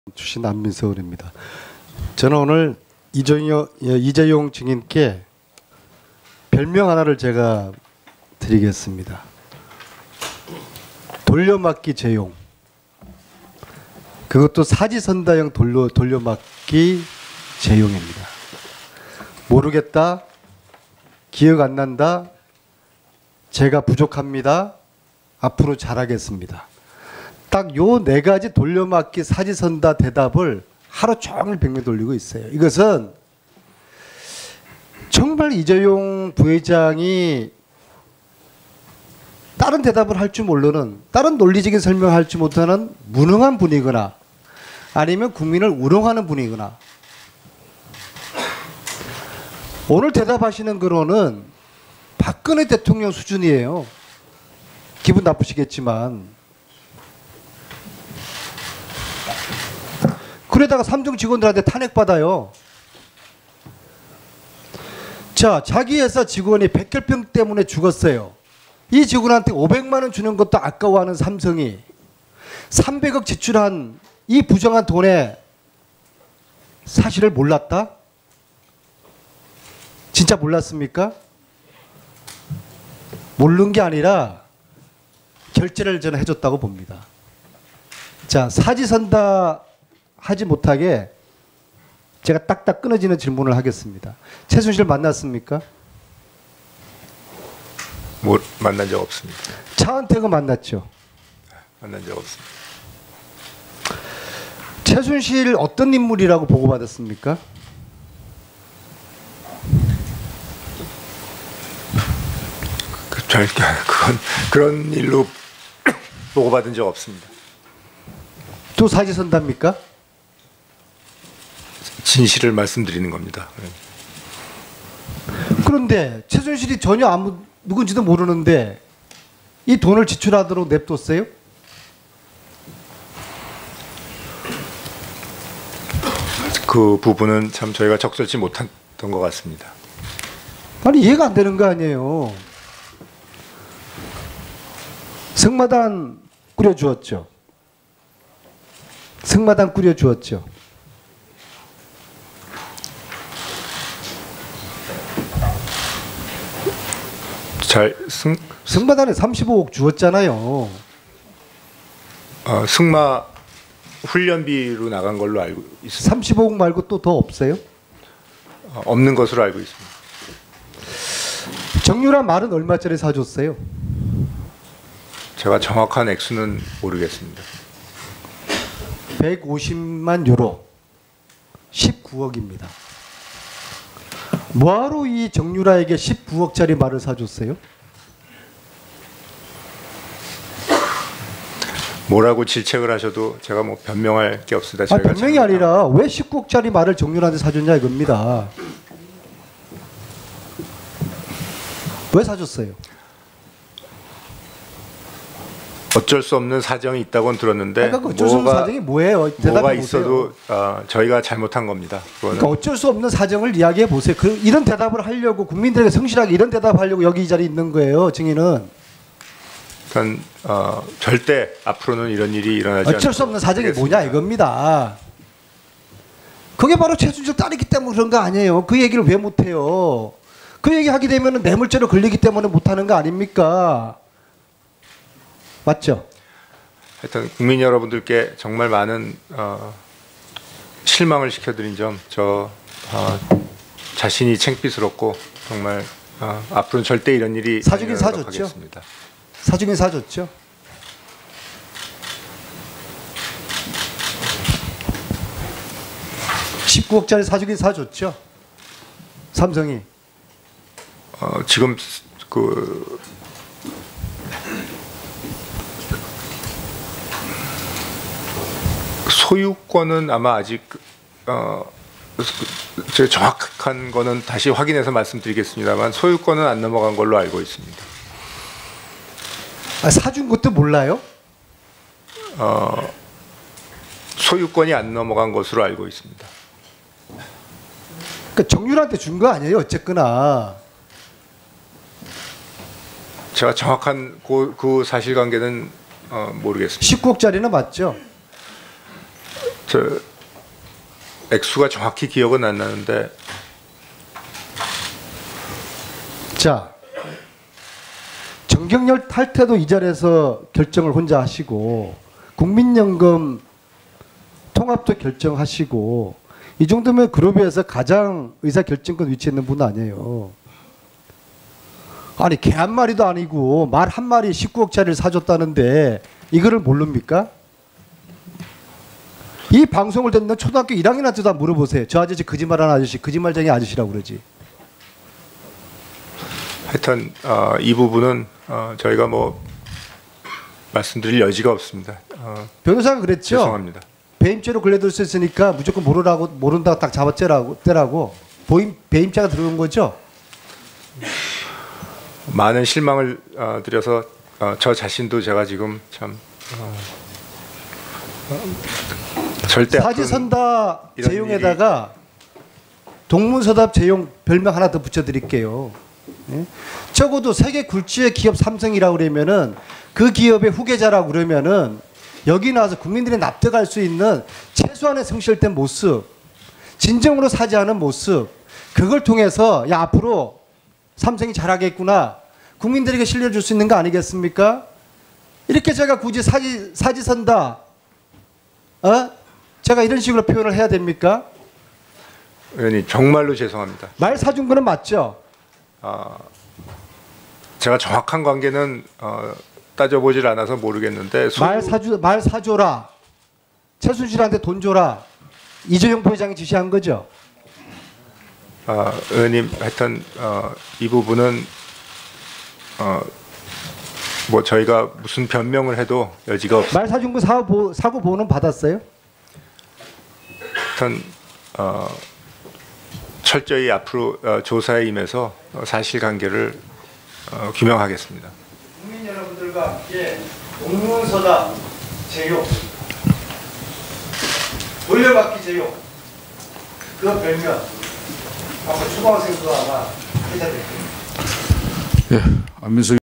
안민석 의원-"사지선다 재용,돌려막기 재용,동문서답 재용" 불성시한 답변 질타
설명안민석 의원-"사지선다 재용,돌려막기 재용,동문서답 재용"  불성시한 답변 질타